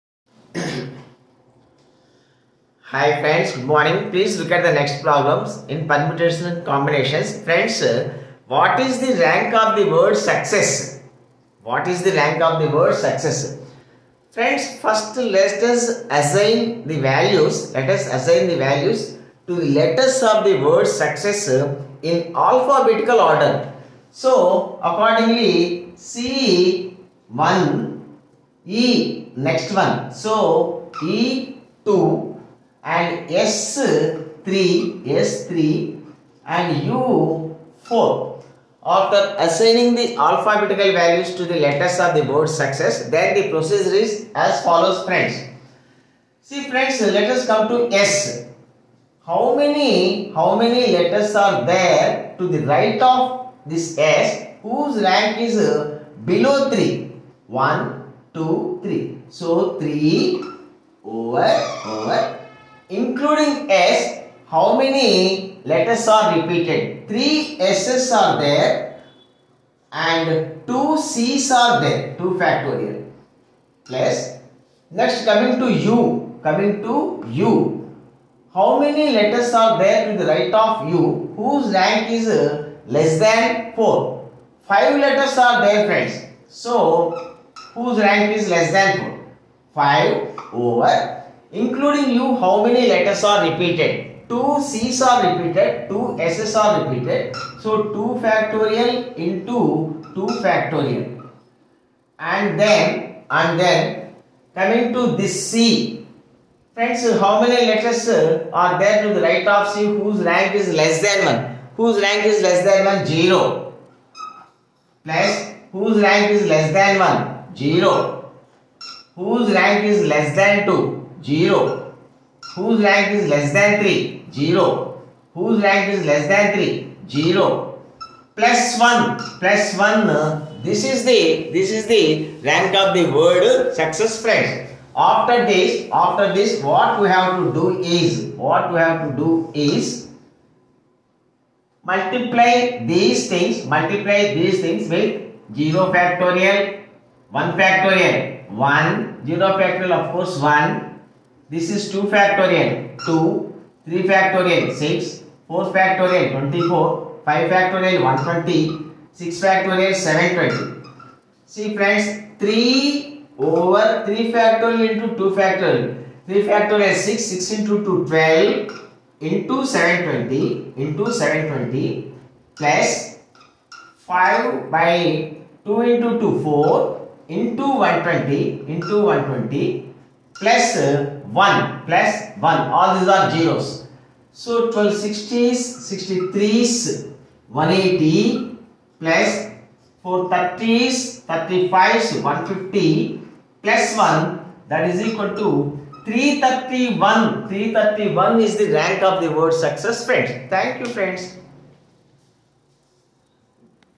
what is the rank of the word SUCCESS
The rank of word 'SUCCESS' in the dictionary if all the words are written as in a dictionary is